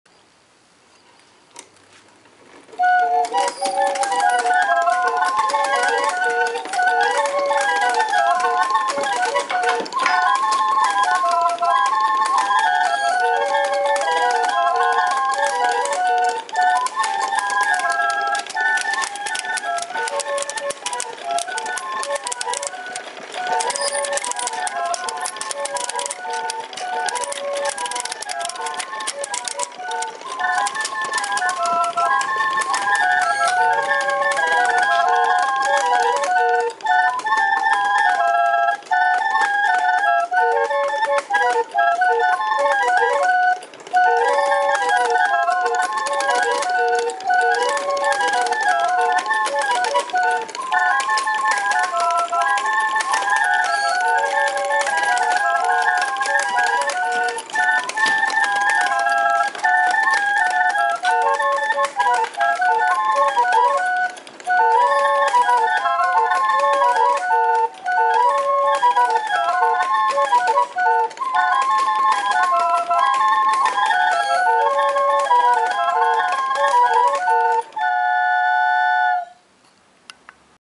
Studio801-13 First Hearing of 4th Organ
This is the first time pipes have been installed and can be heard in this clock organ. Though it might be a bit fast, off key, and mis-voiced, It IS now playing, That is a far cry from what came into the shop to be fixed.  Further refinements are in the works and progress is being made. This instrument was ruined by Silicone. Silicone was used on the wood pours of this organ chest as a sealer. The vintage parts could not be repaired as glue will not stick to silicone. Nearly all of the vintage wood parts have had to be replaced. The vintage parts have been relegated to the status of patterns.